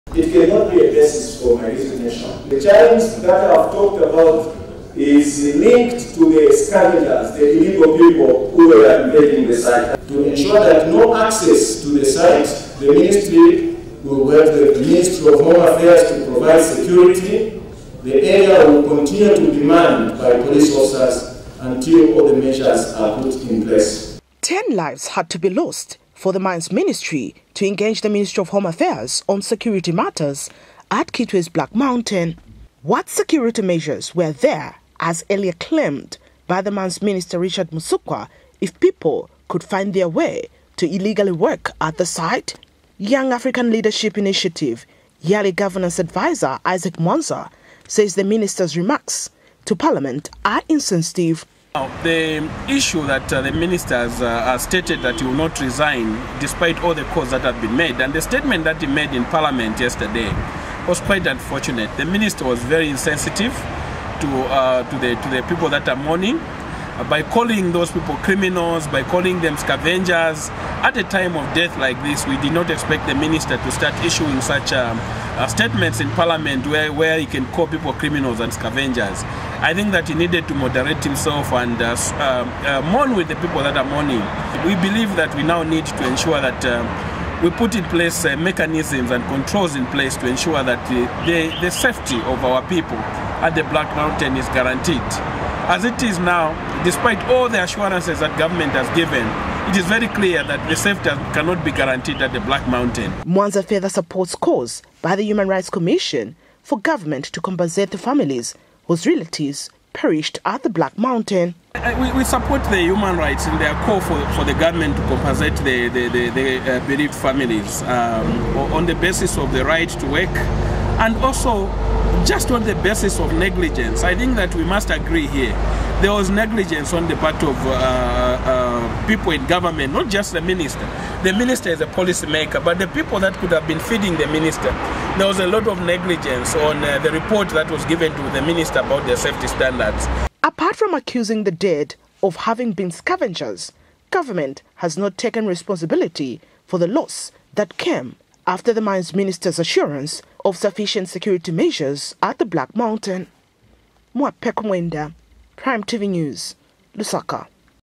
MWANZA ON MUSUKWA
black mountain disaster as miners are killed weeks after safety assurances from government